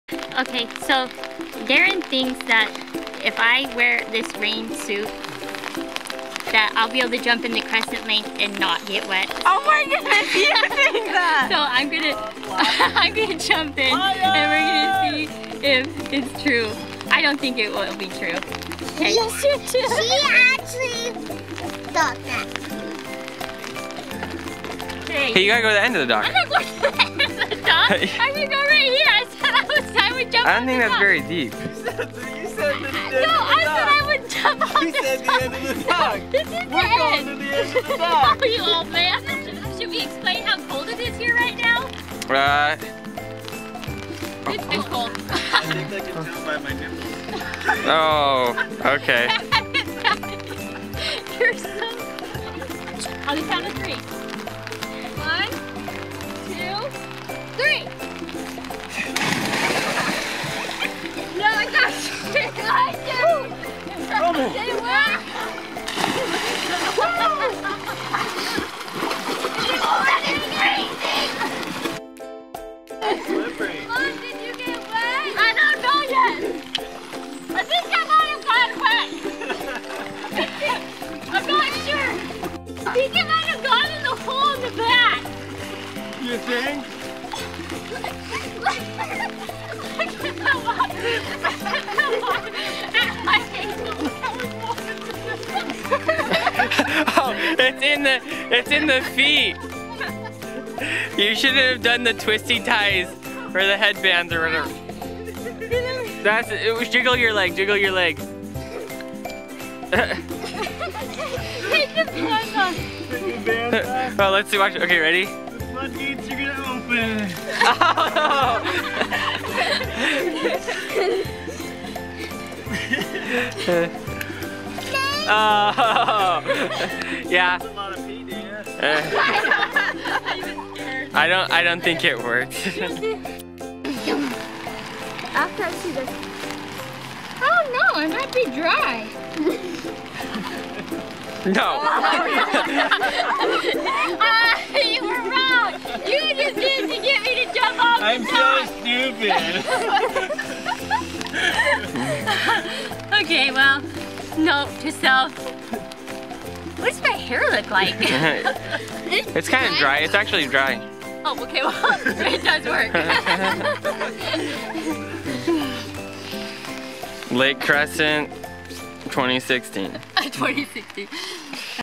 Jones'n Lake Crescent
Being from Arizona, my mom never heard of rain pants.  She thought they were the funniest thing and believed that if she wore them and secured the ends she could jump into Lake Crescent and not get wet. Her experiment follows.